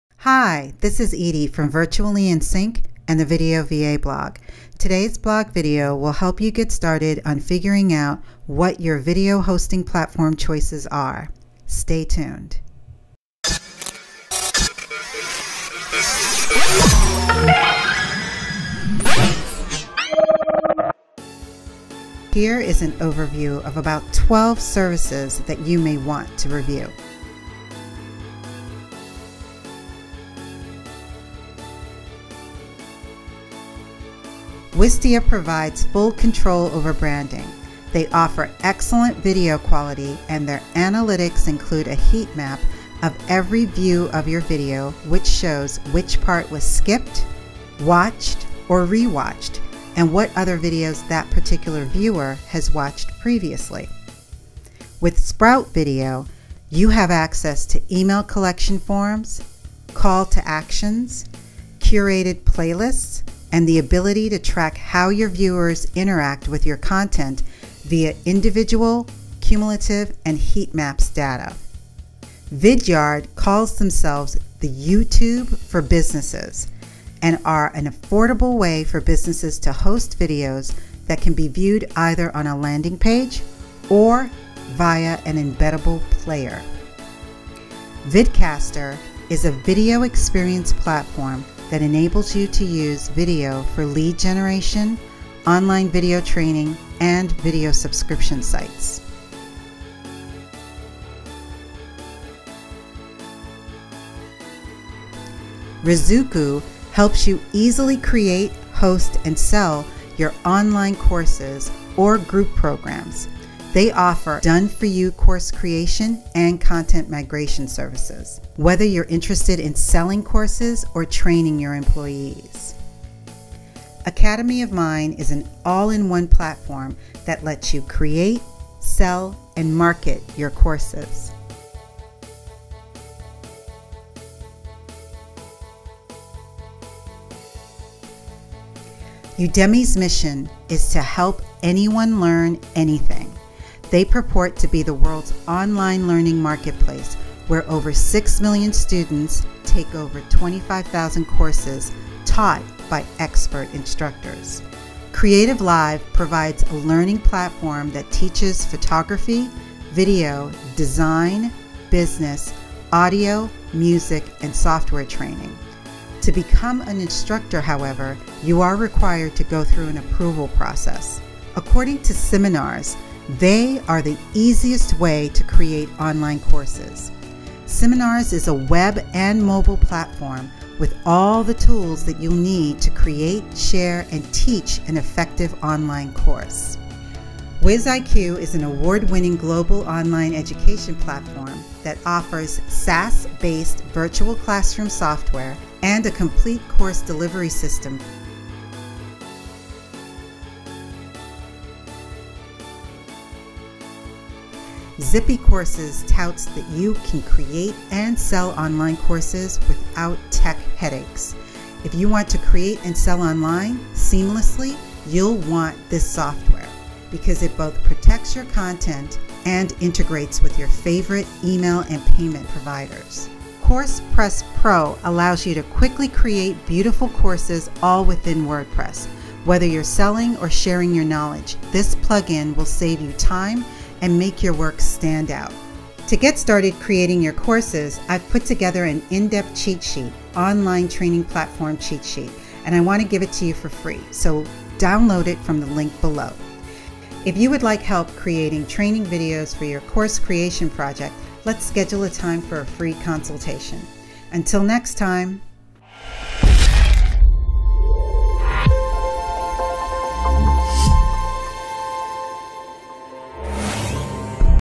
How to Host Your Online Training Videos
Are you currently looking at ways to create an online training program for your new video course? Besides the headache of actually creating the program, are you clueless on where to host it?

Well this blog video will help you get started on figuring out what your video hosting platform choices are.Online training videos are among the most popular and fastest growing trends of the year. Due to this demand, there are numerous websites that provide services for hosting these videos.

There are several factors involved when deciding which is the best video hosting platform for your needs. Most importantly, you will need to decide whether you need a video hosting site or service,(a place where your videos will live) an online training platform (a platform where you can create your courses) or an online learning marketplace (a service that will handle all marketing and hosting, after you've uploaded your training videos.Sometimes it may just come down to your budget.